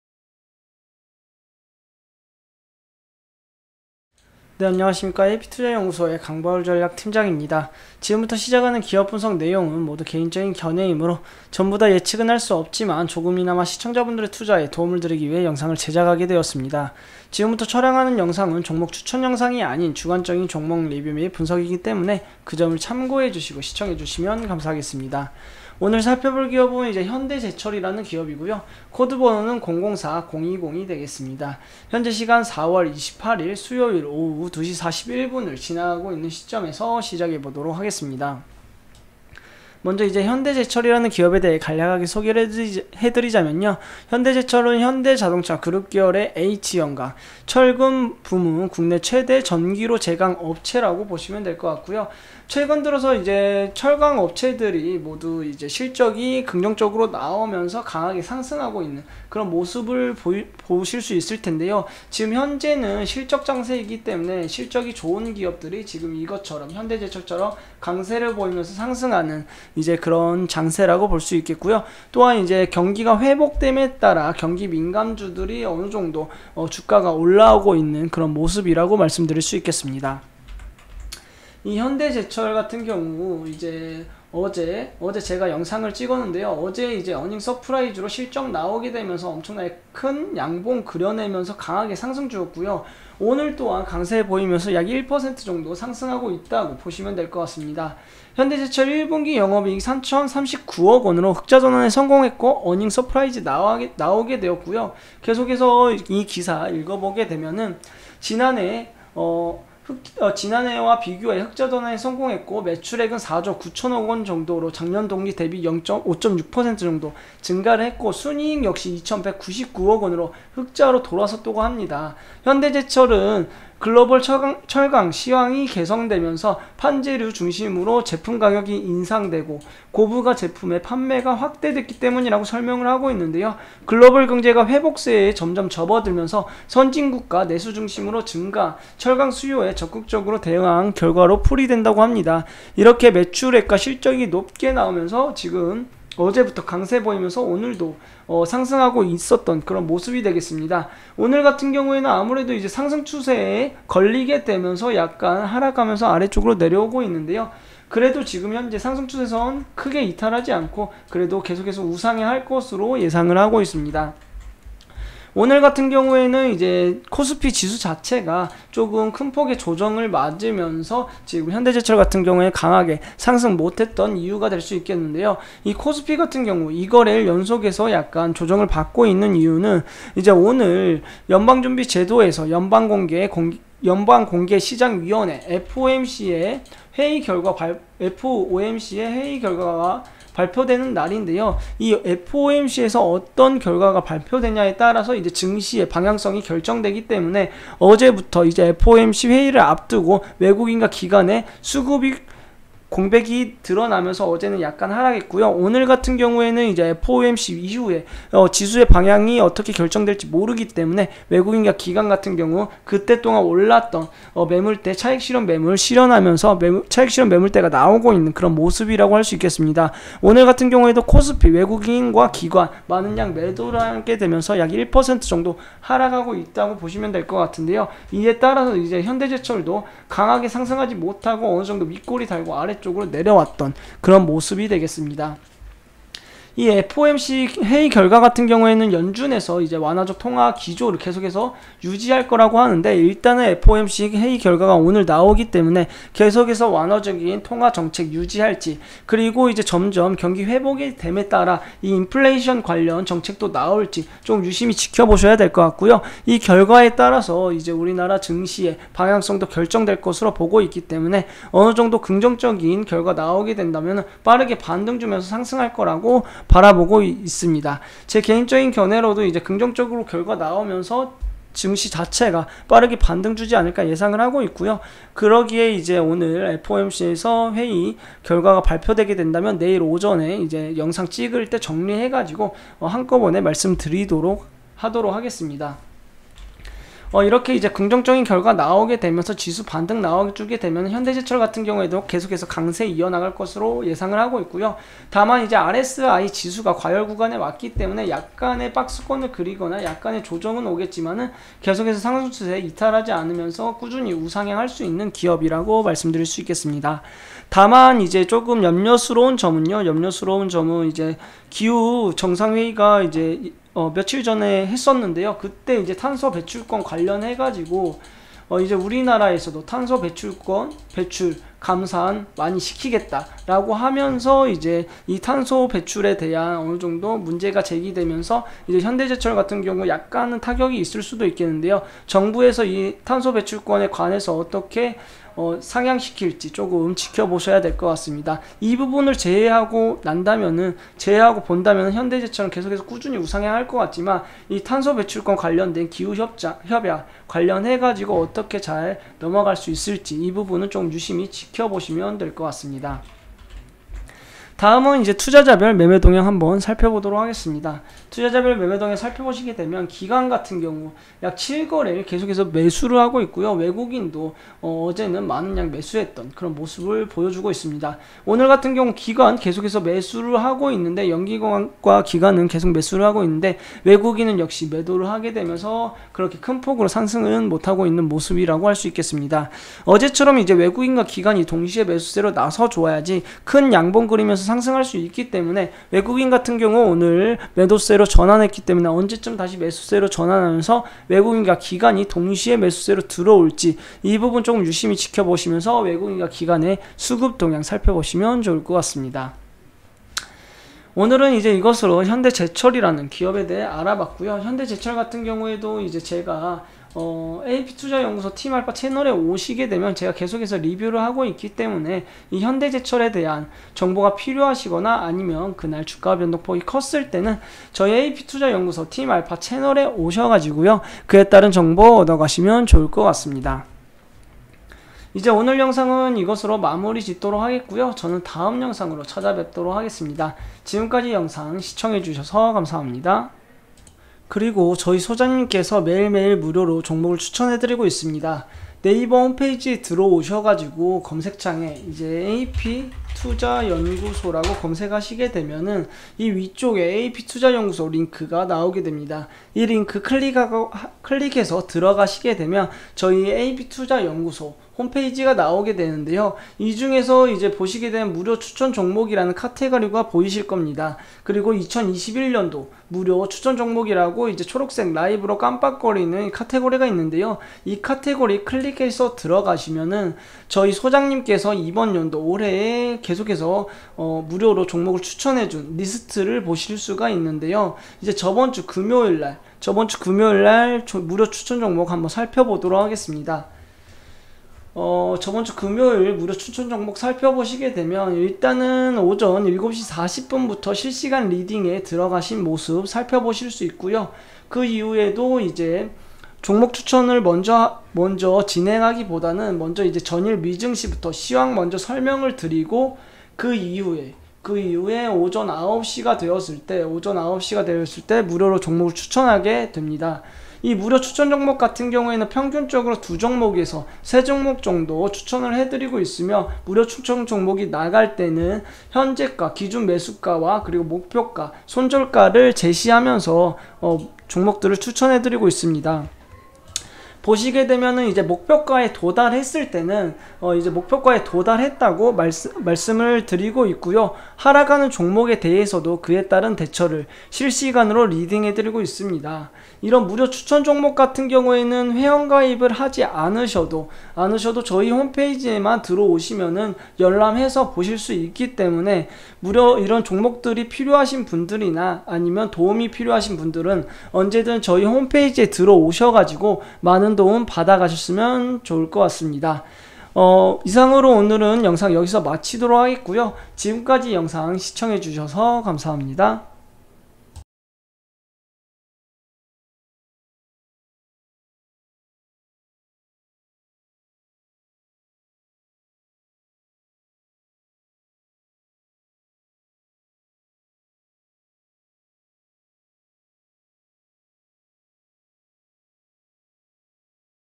현대제철(004020) - 경기민감 산업인 철강산업 계속해서 우상향 순항중~!
김용재소장이 직접 매일마다 홈페이지에서 무료로 종목추천 및
장중 시황을 진행하고 있습니다.